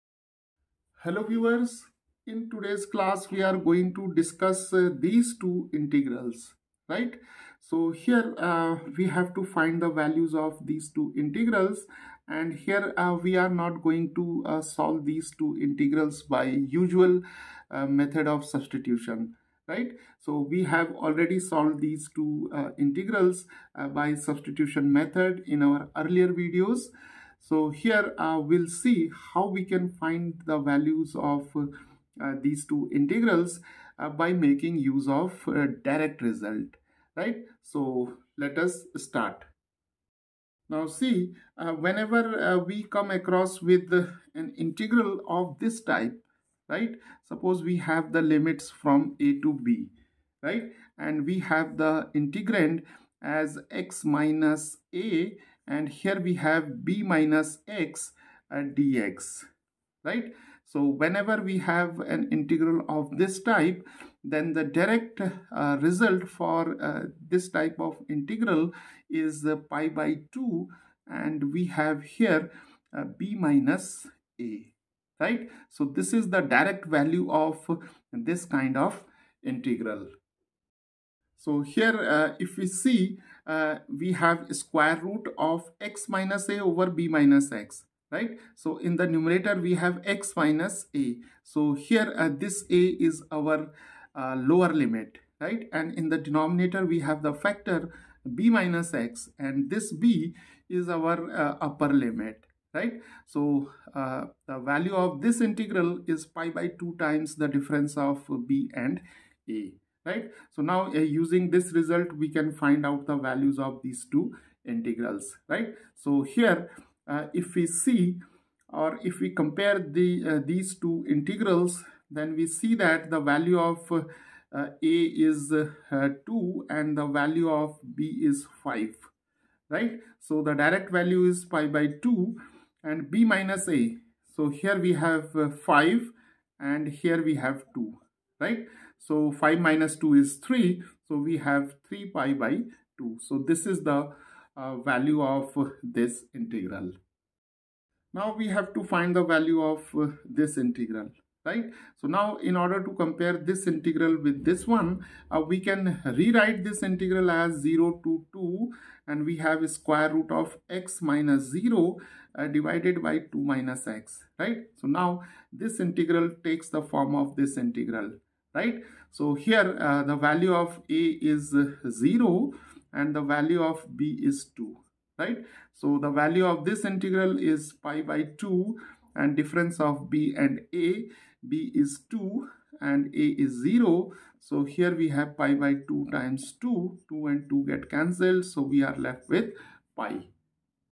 Two Special definite Integrals- Direct Result
How to make use of direct results to evaluate integrals.